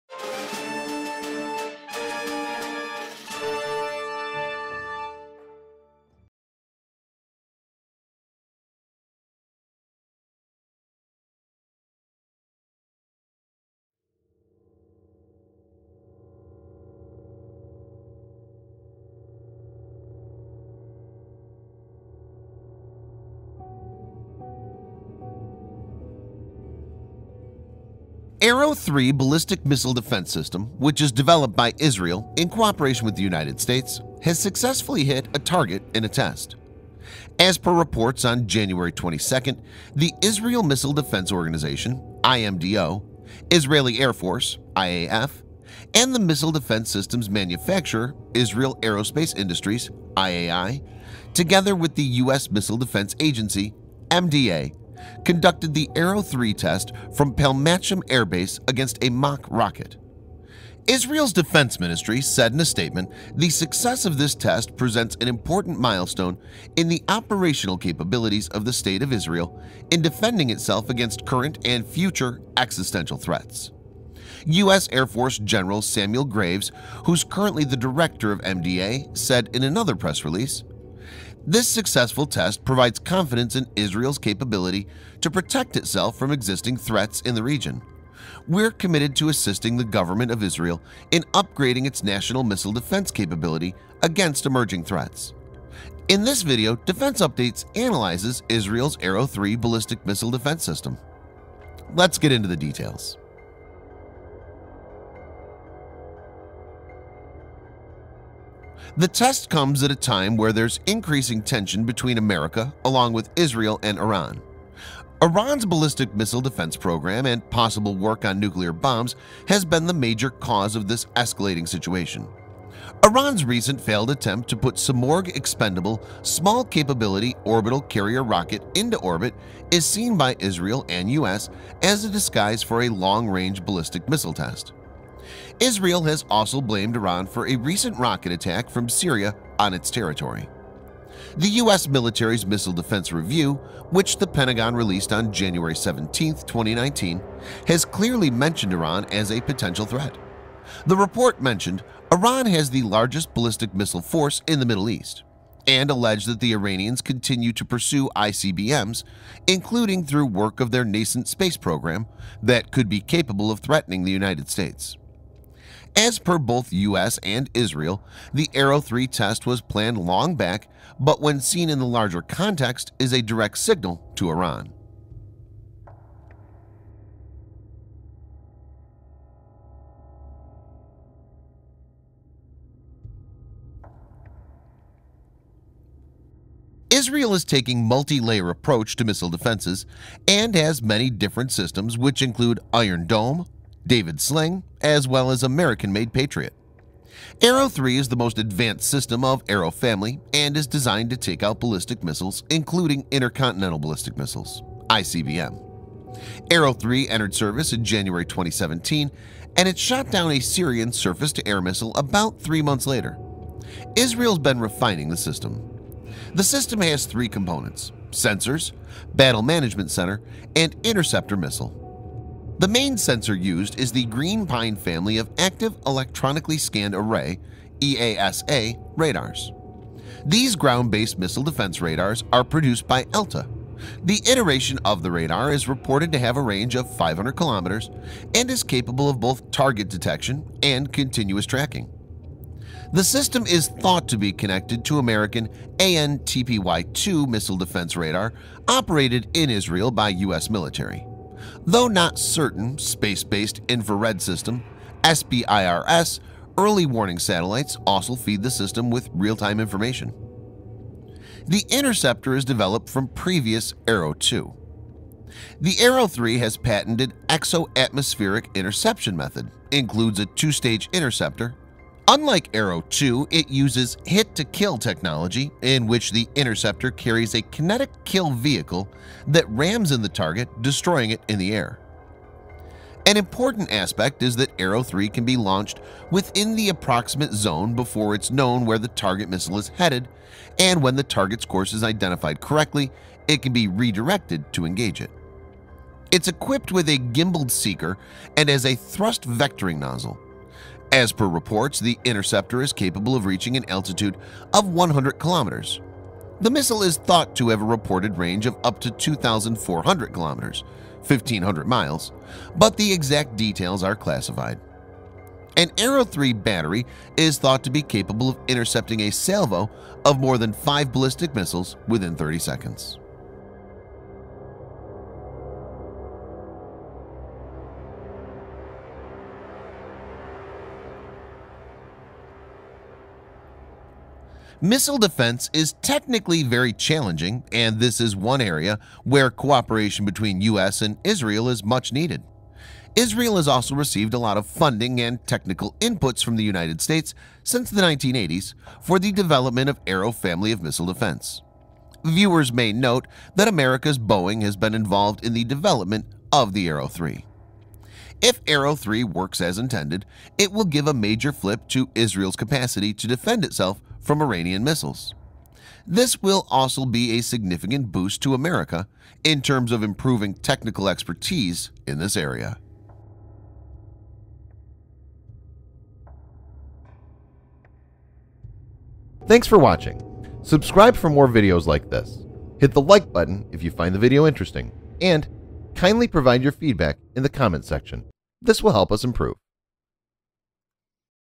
ISRAEL’S ARROW 3 SYSTEM BUILD WITH AMERICAN COOPERATION HITS BULL'S EYE
Arrow 3 ballistic missile defense system which is developed by  Israel in cooperation with the United States, has successfully hit a target in a test.

As per reports on Jan 22, the Israel Missile Defense Organization (IMDO), Israeli Air Force (IAF) & the missile defense system's manufacturer Israel Aerospace Industries (IAI), together with the U.S. Missile Defense Agency (MDA) conducted the Arrow 3 test from Palmachim Air Base against a mock rocket.

Israel’s Defense Ministry stated in a statement,
“The success of this test presents an important milestone in the operational capabilities of the State of Israel in defending itself against current and future existential threats,”

 U.S. Air Force General Samuel Greaves who is currently the director of MDA said in another press release,
“This successful test provides confidence in Israel's capability to protect itself from existing threats in the region. We are committed to assisting the government of Israel in upgrading its national missile defense capability against emerging threats.”

In this video Defense Updates, analyzes  Israel’s Arrow 3 ballistic missile defense system.

Let's get into the details.

BACKGROUND MUSIC

IMAGES USED